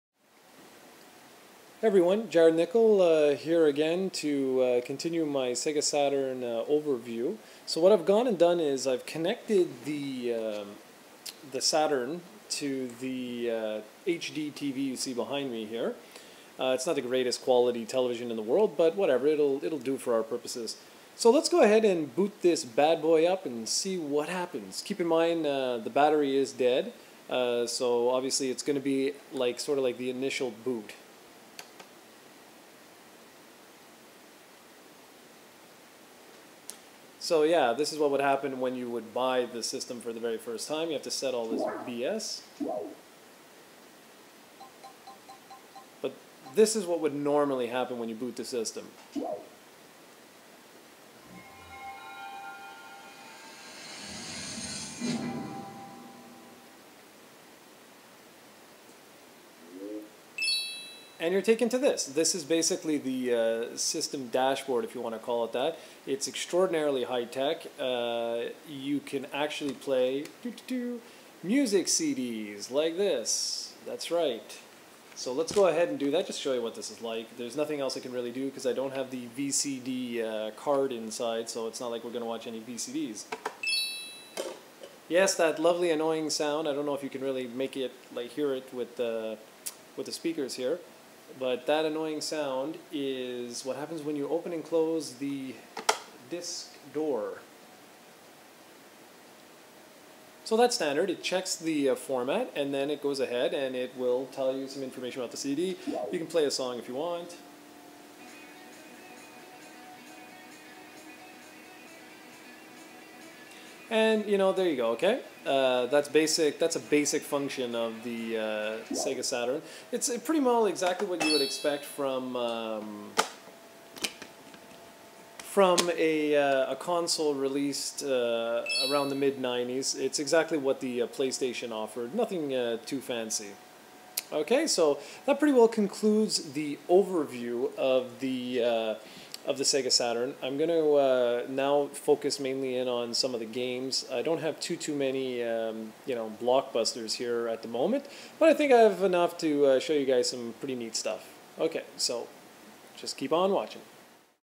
Sega Saturn Overview - Part 2
Jarrod now shows you what happens when you boot the Saturn for the very first time.  The dashboard is kind of old-school by today's standard, but hey this way breakthrough stuff back in 1995.